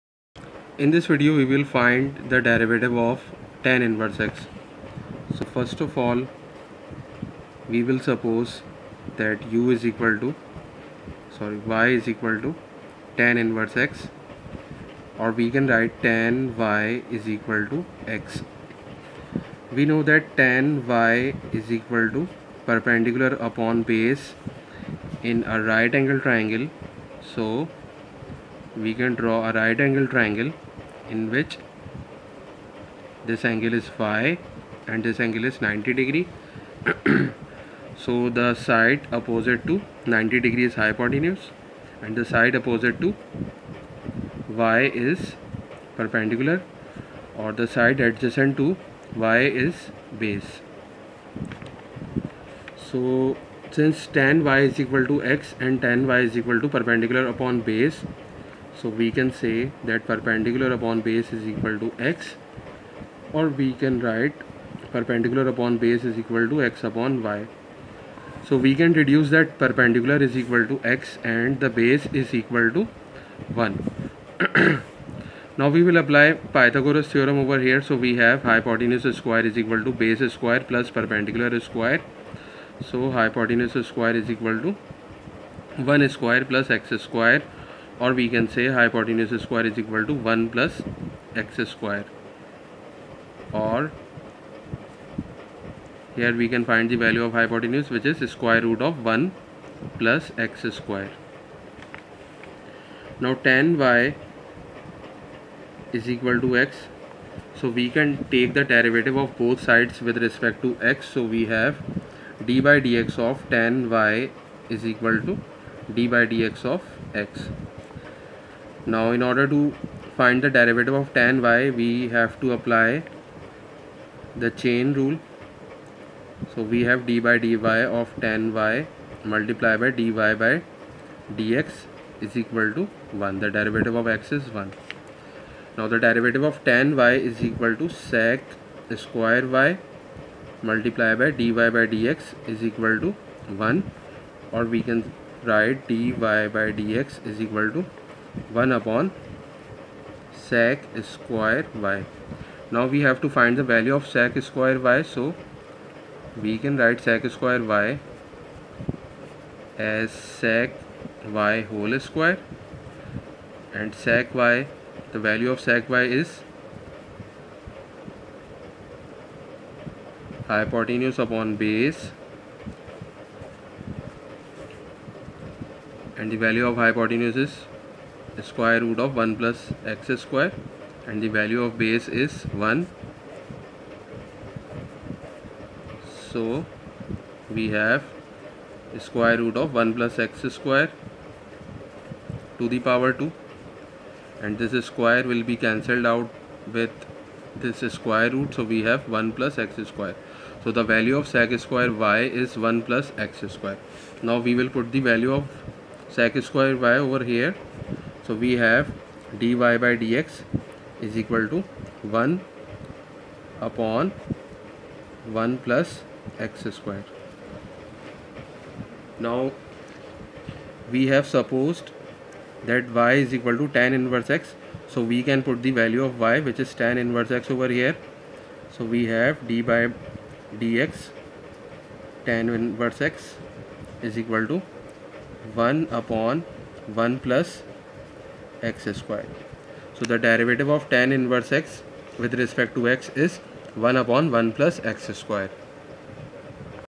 Derivative of tan inverse x || Differentiation of inverse trigonometric functions
In this video we will find the derivative of tan inverse x.